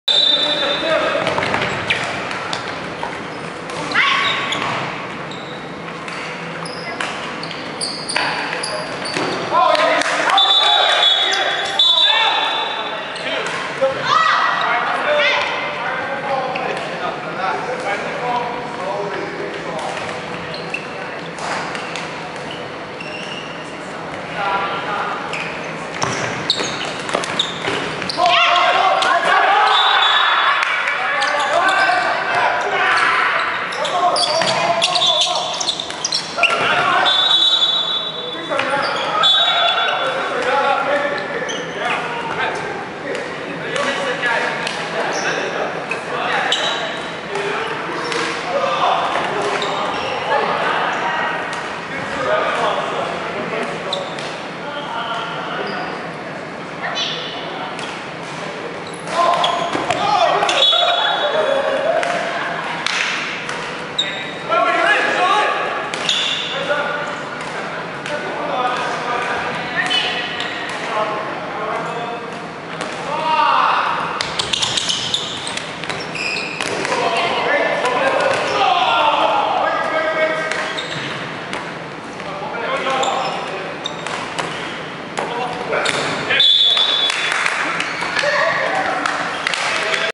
Boiling Guys vs Ballgazers Set 14 of 17
Boiling Guys win set, score 10-4